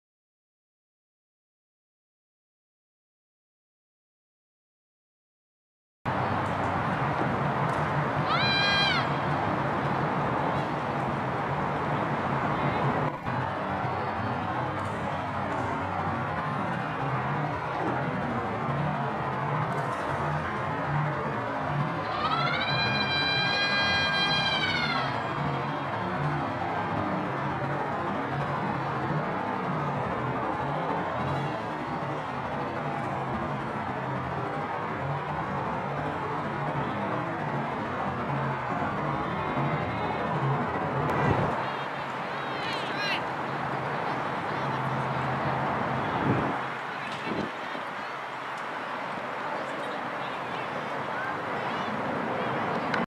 Firecracker 1st Base Foul Ball Dive into Fence @ Softball Tournament, Surf City, CA. Emily Burrow
Emily Burrow playing up for SoCal Firecrackers 18u Gold vs OC Batbusters @ Surf City, CA Softball Showcase. Fastpitch Travel Softball Played on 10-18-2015.  Emily Burrow Class of 2017. Varsity Coach: Dave Mercado at Great Oak High School, Temecula CA.